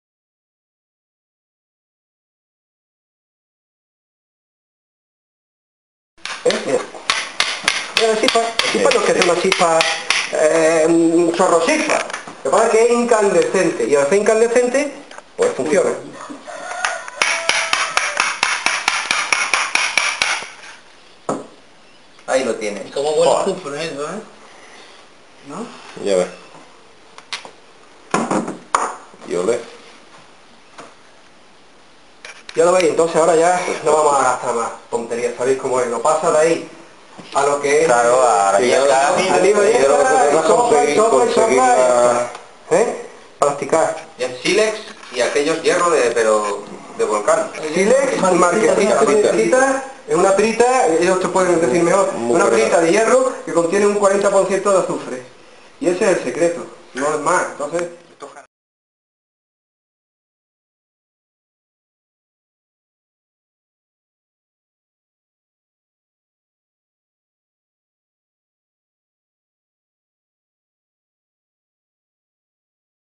Fuego por percusión silex marcasita
Fuego por percusión Silex contra Marquesita

El sistema de percusión para hacer fuego, es uno de los más antiguos que existen. En este caso utilizamos 'raspadura' de hongo yesquero (fomes fomentarius) y para percutir, Silex contra Marquesita (que es una pirita). Este proceso es conveniente realizarlo en una zona donde haya poca luz y nada de viento, de esta forma podemos apreciar la caída de la chispa. Al no haber viento, no la desplazara de nuestro objetivo. La marquesita contiene un alto porcentaje de azufre. Este mineral es el responsable de que la incandescencia de la chispa dure más tiempo y de esta forma sea posible iniciar el encendido de nuestra yesca.